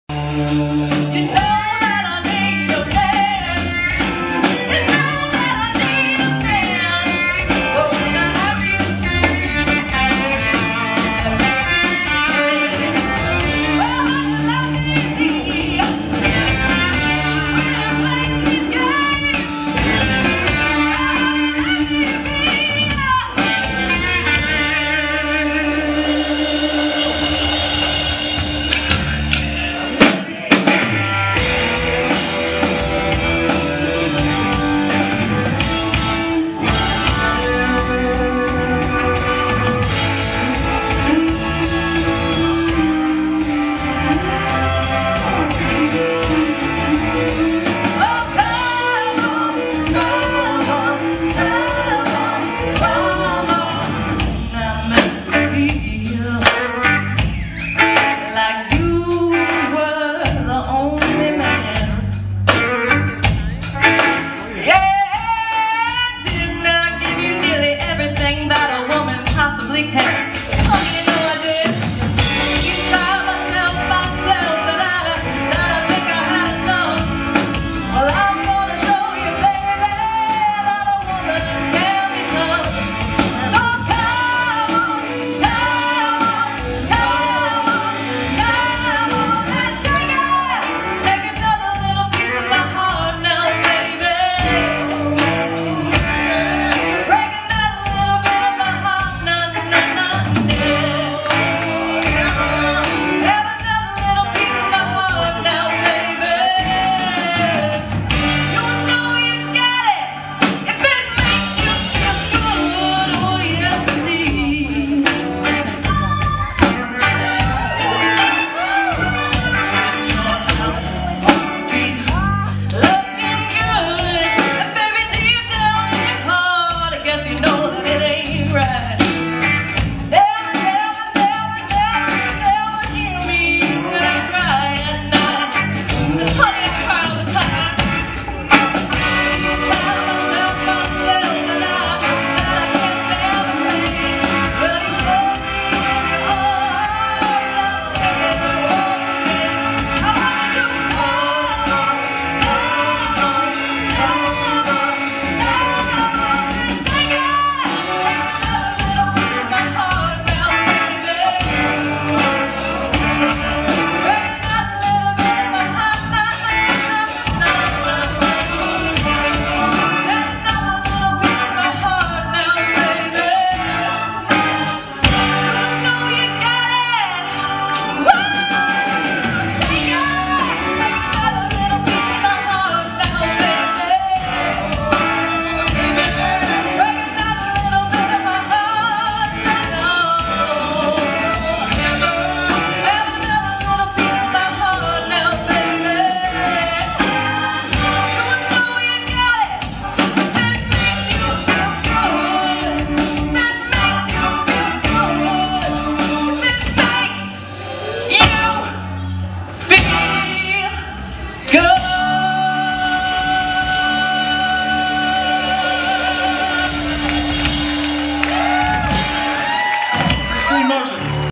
"Soul of a Legend" Band - "Christine" singing Janis Joplin
Not sure of the singer's name, Christine?  It was a good show at The Regent Theater  Arl, Ma Sept 11, 2021.  "Christine" can really belt out Janis Joplin. She was great. More coming of "Soul of a Legend" nite. Hope you like it.